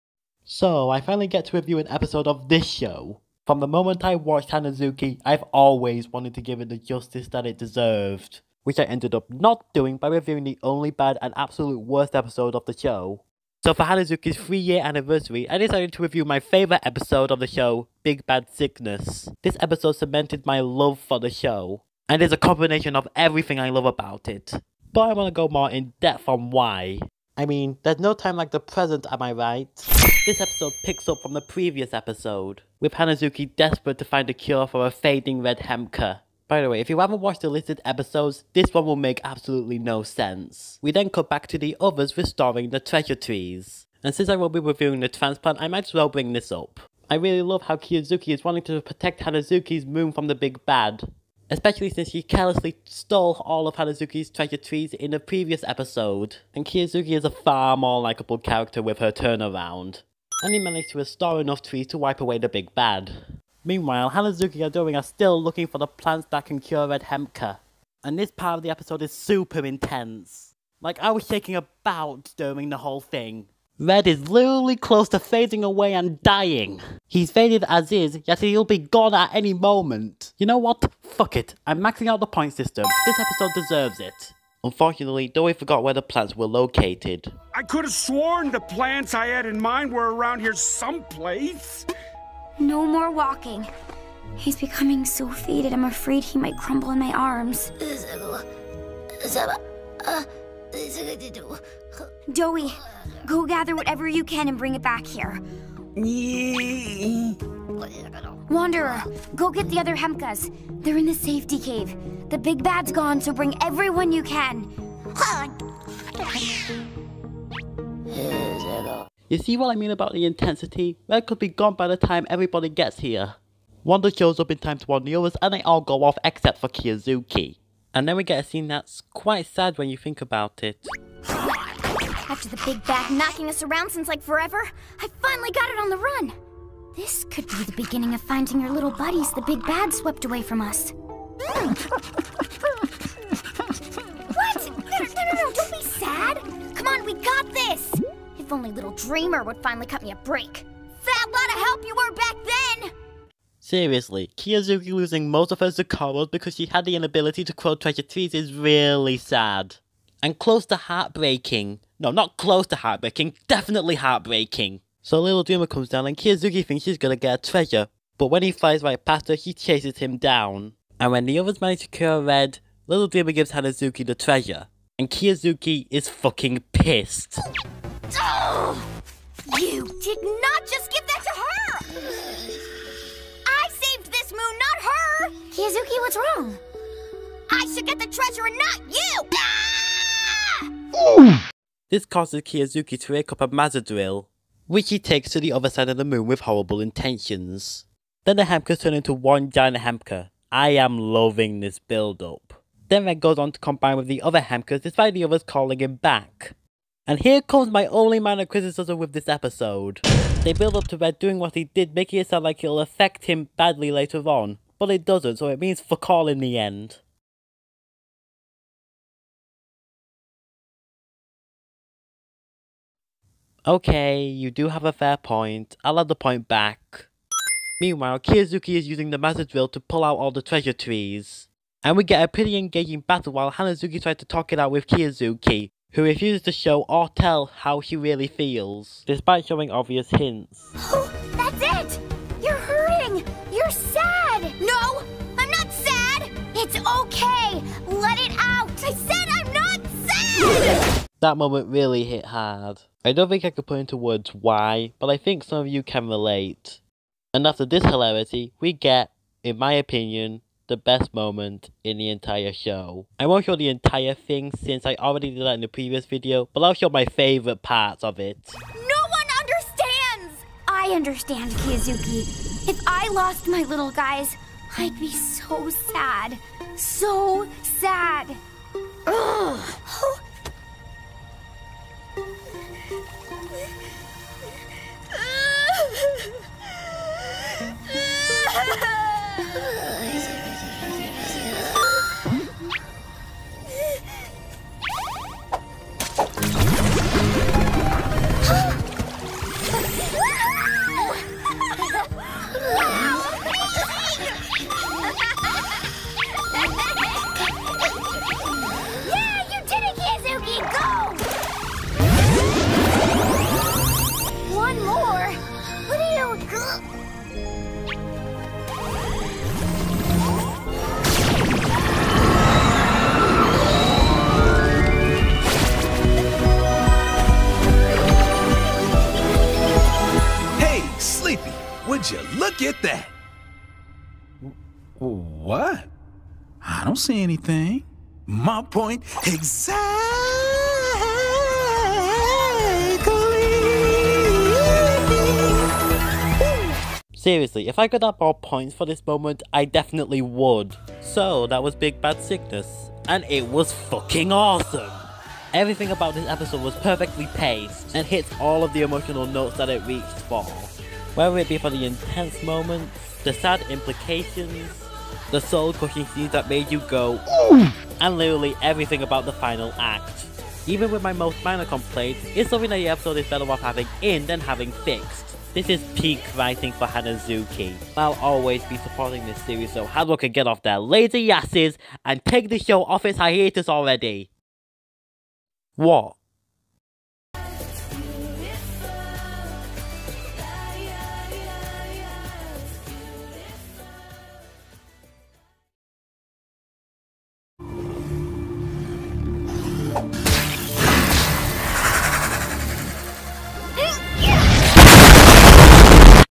Hanazuki - "Big Bad Sickness" - Episode Review
I can't believe it took me this long to get the video finished. Anyways, time for me to review my favourite episode of one my favourite cartoons ever made.